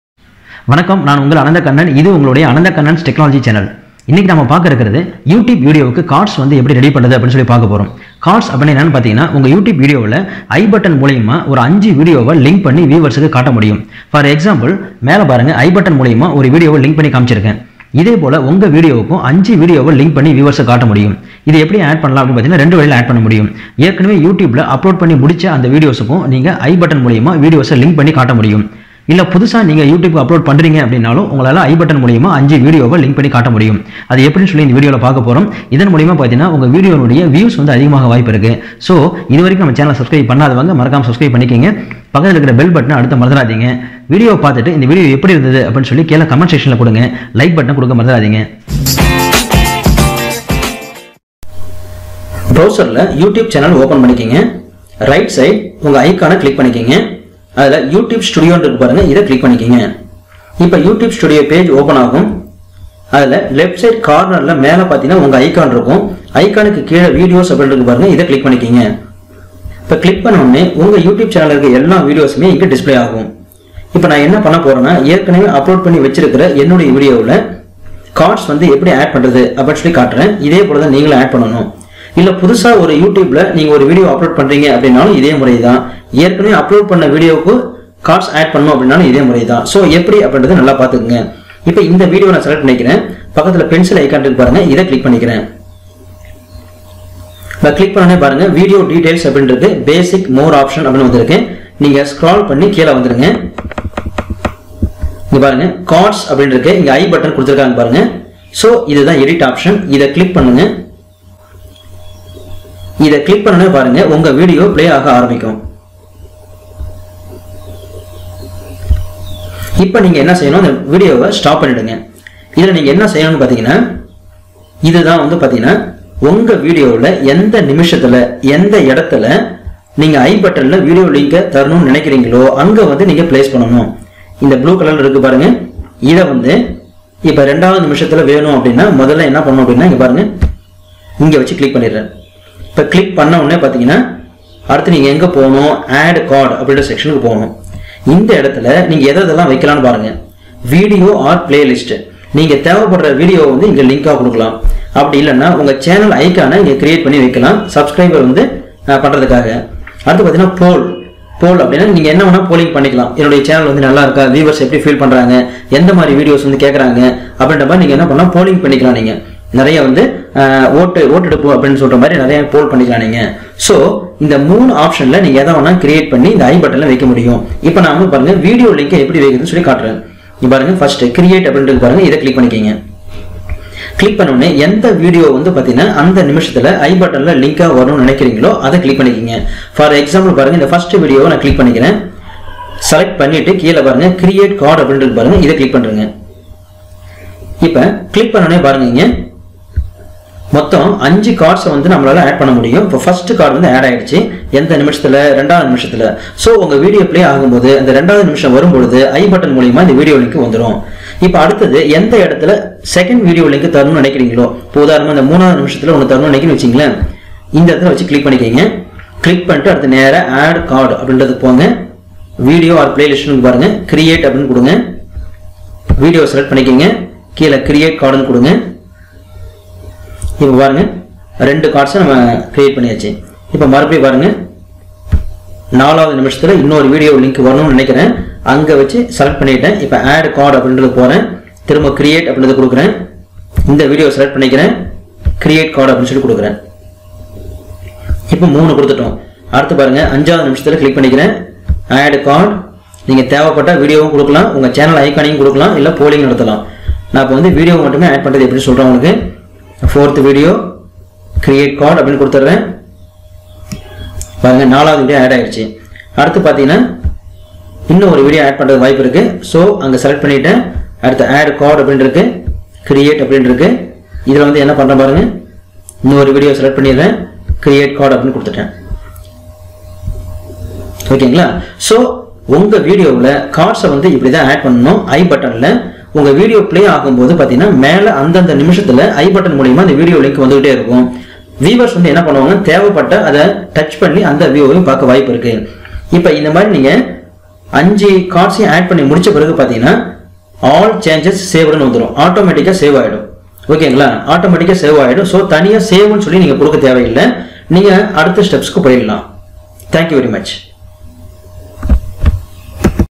How to Add “ i cards” To Youtube Videos – YouTube வீடியோவில் i cards சேர்ப்பது எப்படி
Type 1 – ஏற்கனவே Upload செய்து, உங்கள் சேனலில் உள்ள வீடியோ அனைத்திற்கும்  i cardsசேர்க்க முடியும்

Type 2 – புதியதாக You Tube ல்,Upload செய்யும் போதும் i cards சேர்க்க முடியும்